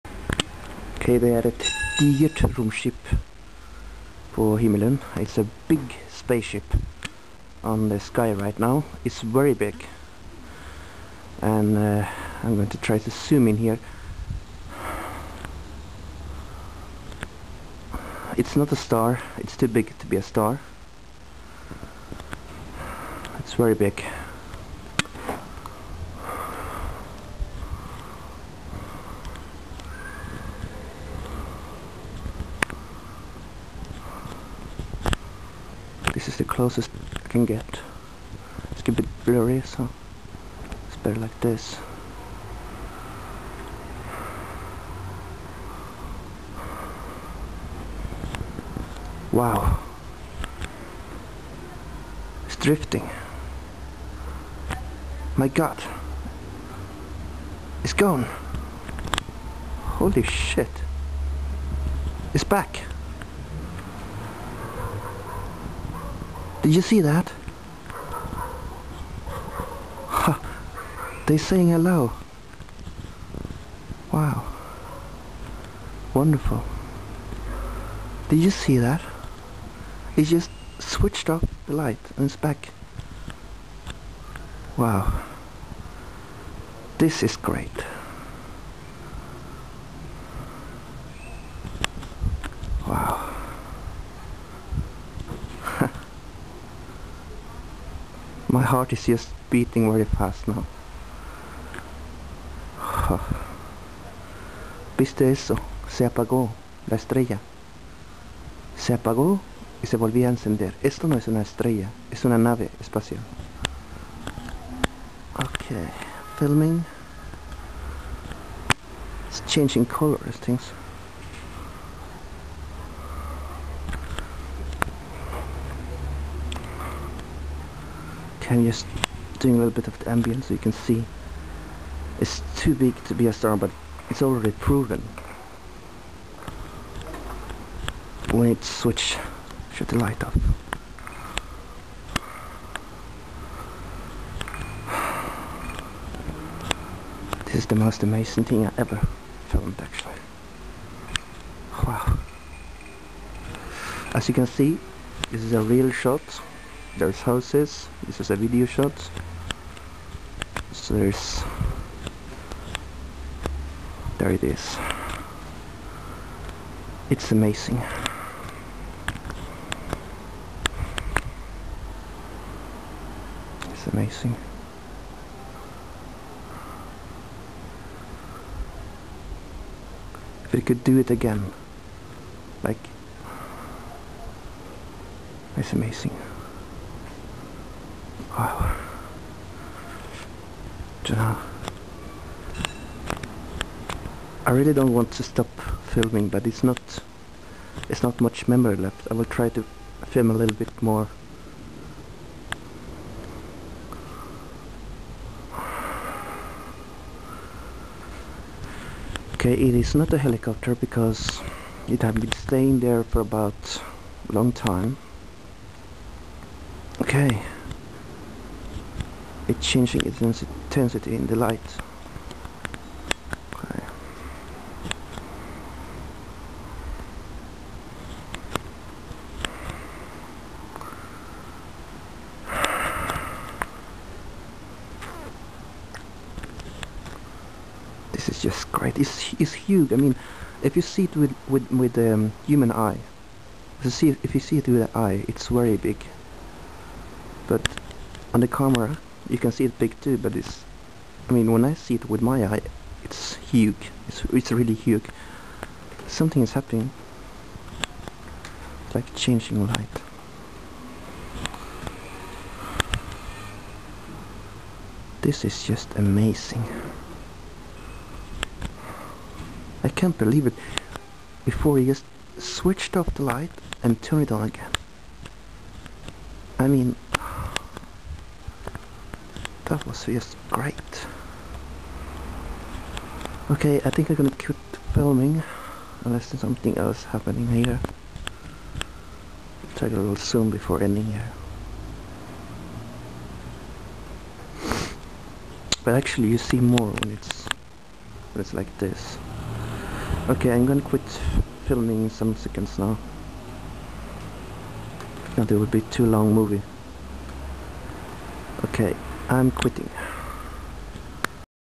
When The Stars Disapear
Watch As the Star suddenly TURN Off and On, Hey wait when did the stars began doing that? Could this be the First video ever of a Artificial star observing us.